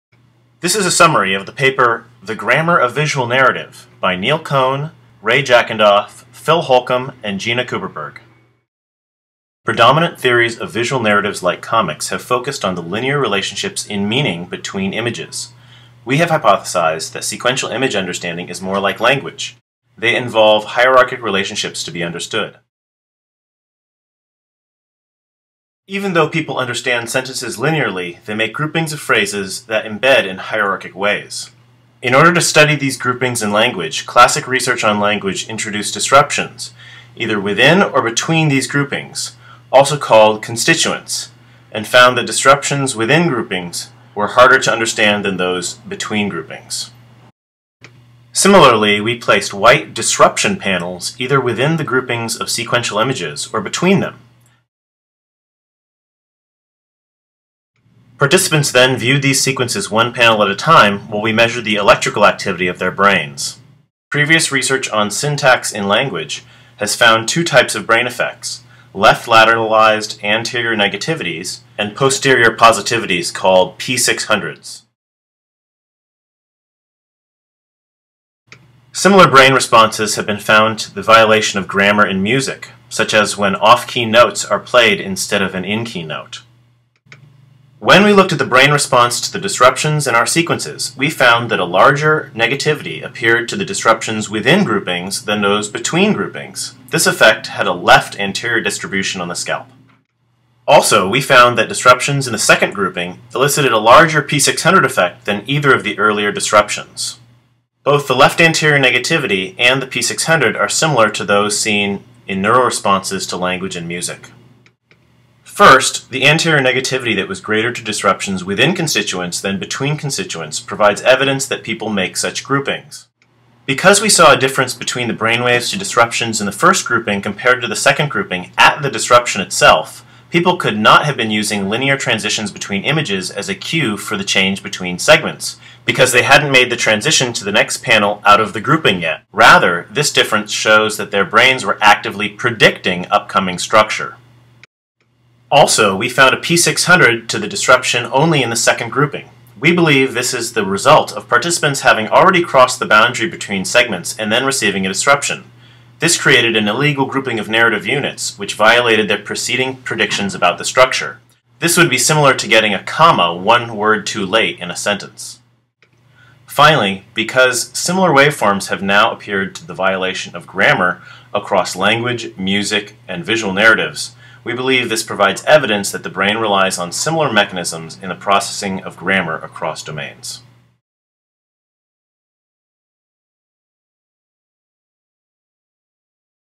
The grammar of comics in the brain
A summary of the paper:

Cohn, Neil., Jackendoff, Ray., Holcomb, Phil. J., & Kuperberg, Gina. R. (2014). The grammar of visual narrative: Neural evidence for constituent structure in sequential image comprehension. Neuropsychologia, 64, 63-70. doi: 10.1016/j.neuropsychologia.2014.09.018